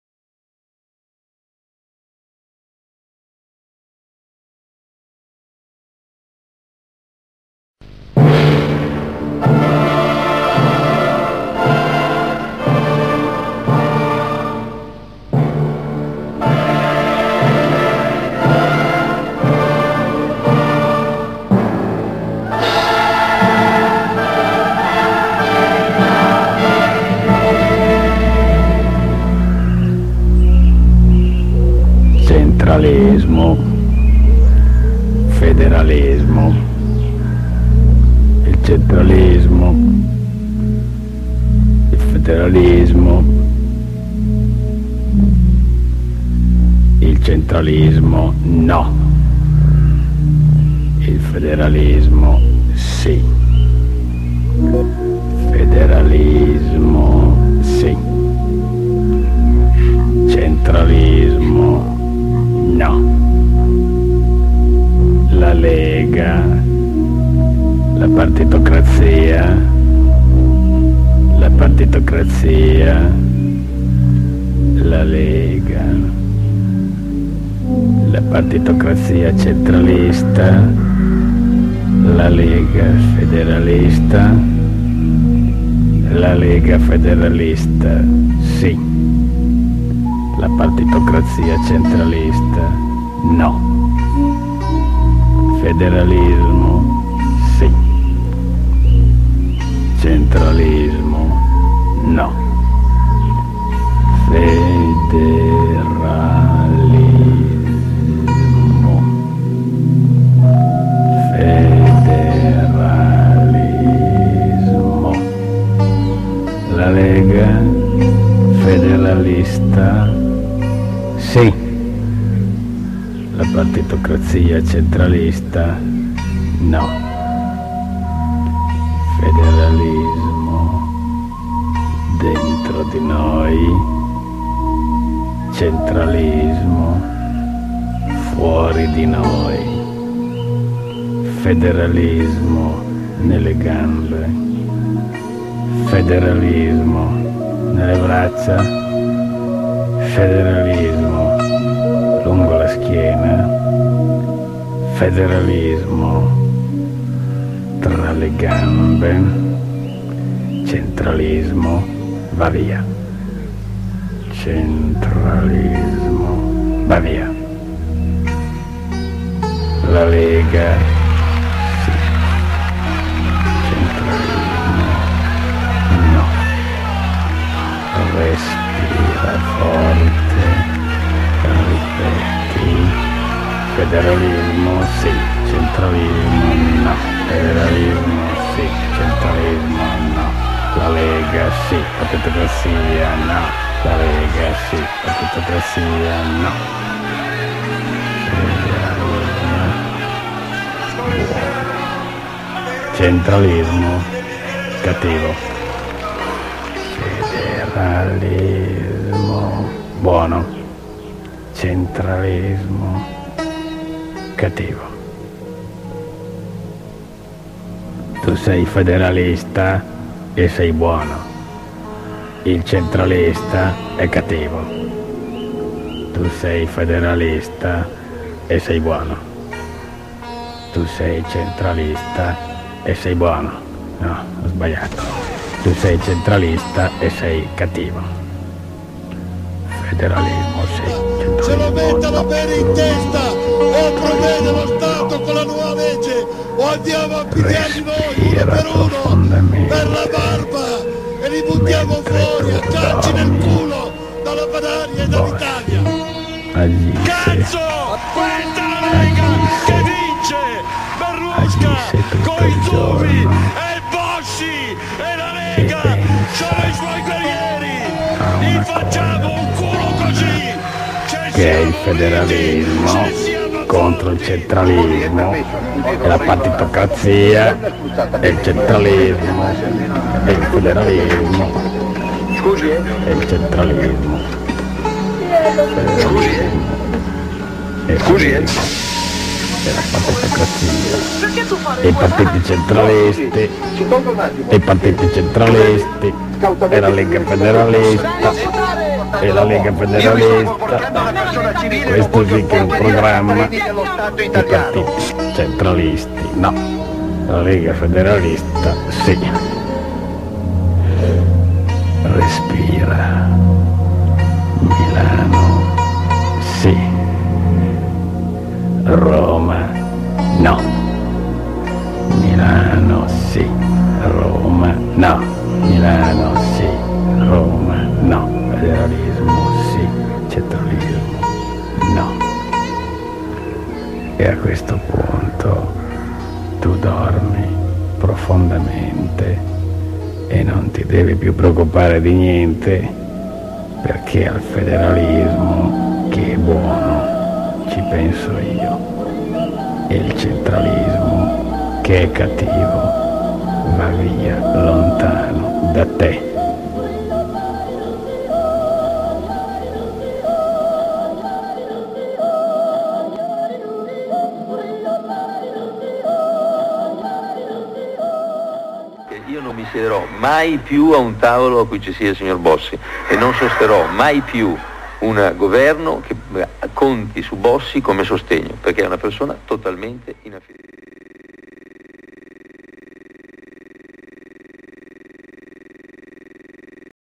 Leghista con l'ipnosi - Arte e Creazione
Una libera interpretazione dal famoso giornale satirico "Cuore". Una parodia di qualche anno fa quando la lega faceva solo ridere.